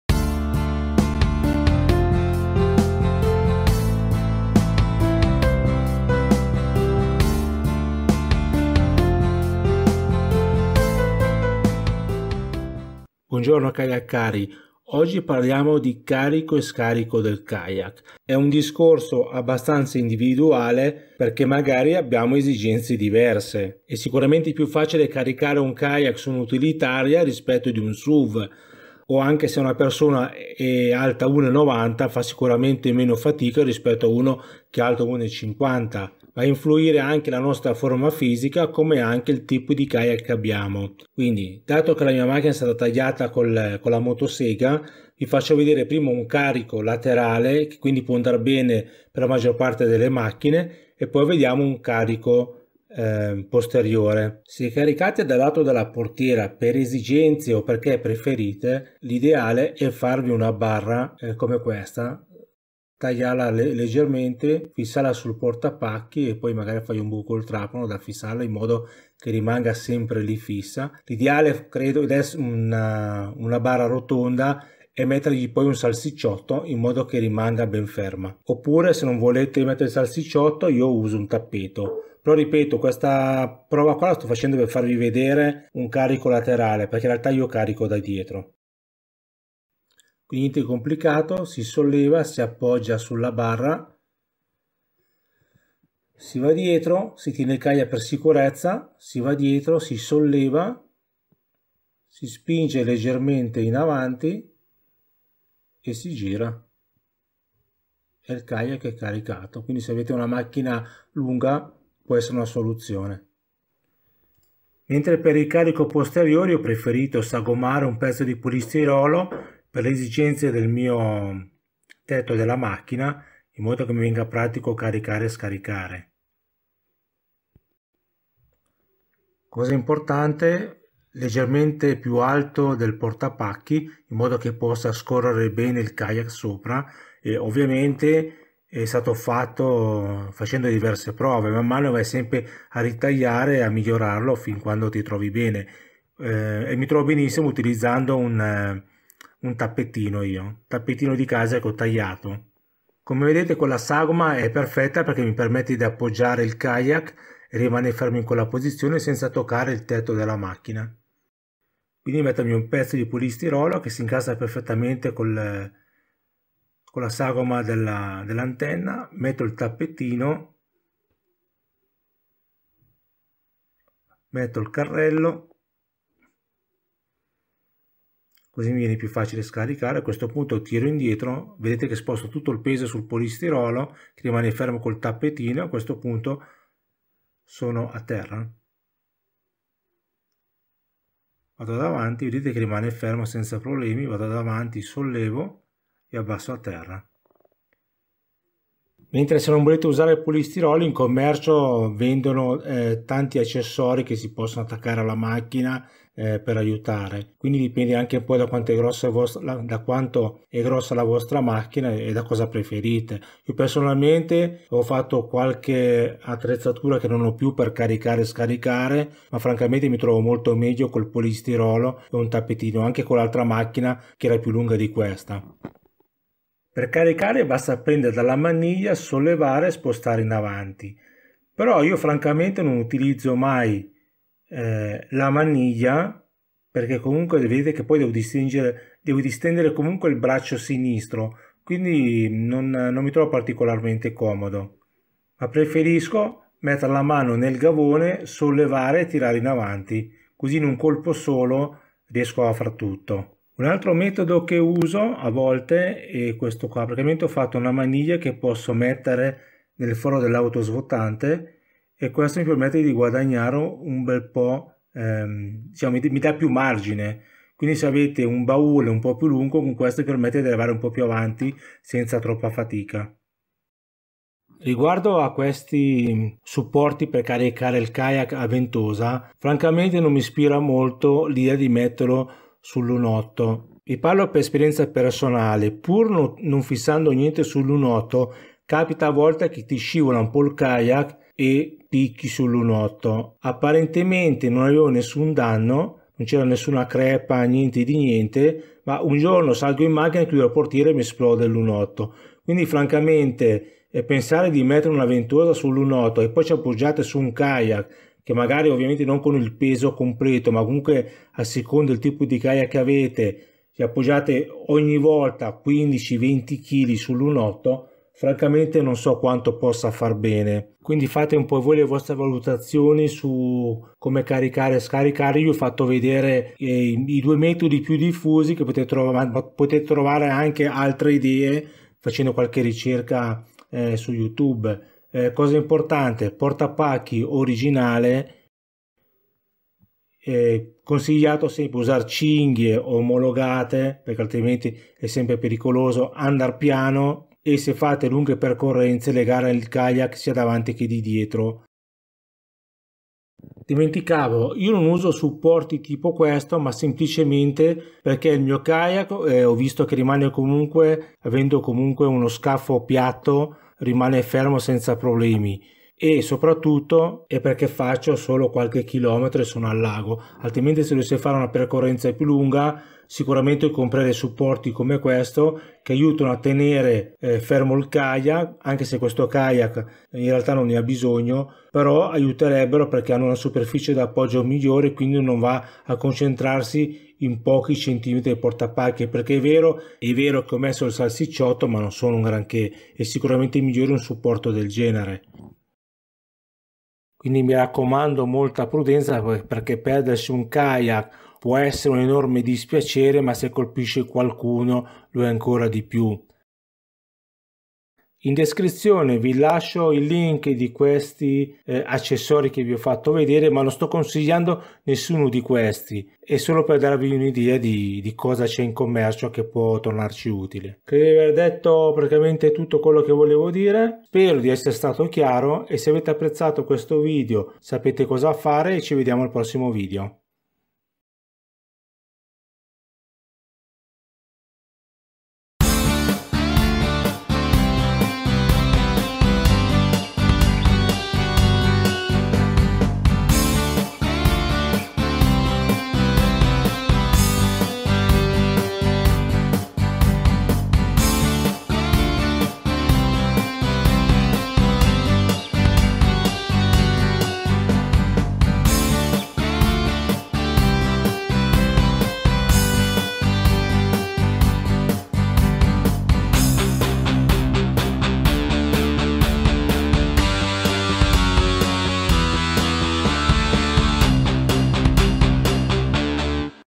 Carico Scarico Kayak con poca fatica
Carico Scarico Kayak con poca fatica,un metodo semplice ed economico per caricare il kayak sull'auto.